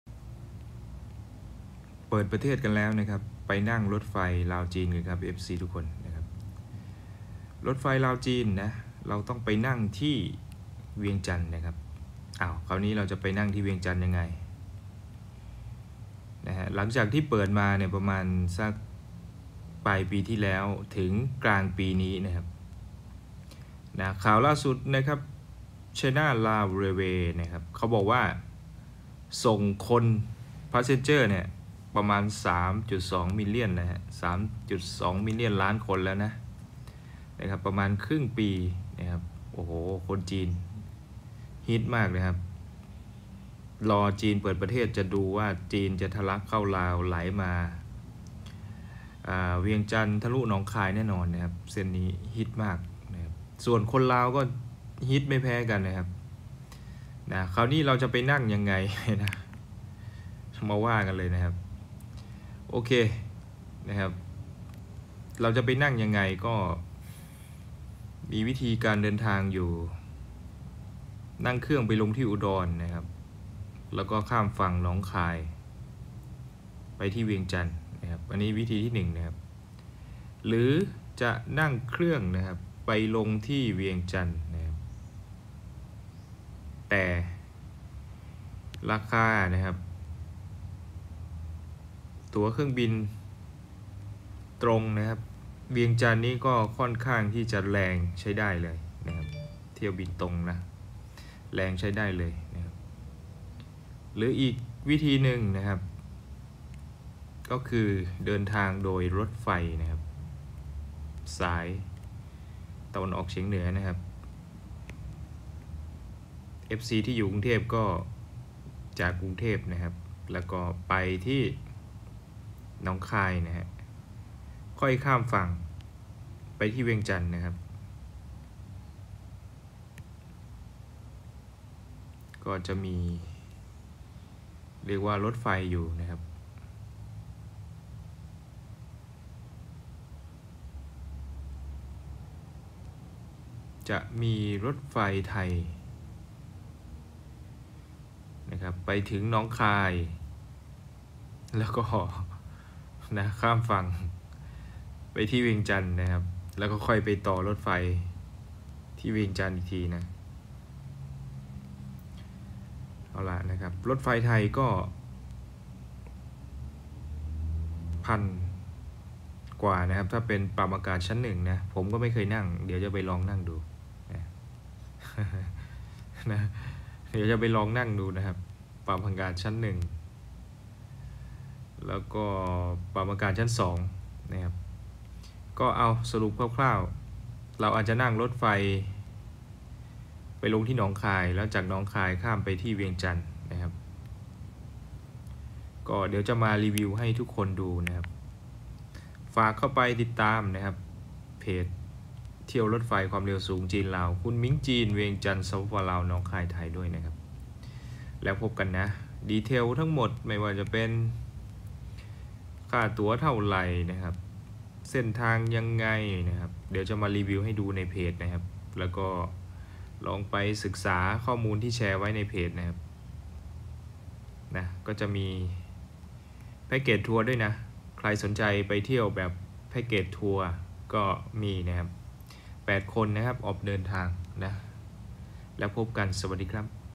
นั่งรถไฟไทย กรุงเทพ หนองคาย เวียงจันทน์ ไปนั่งรถไฟลาวจีน กัน พาเที่ยวลาว หลวงพระบาง วังเวียง
ไปกัน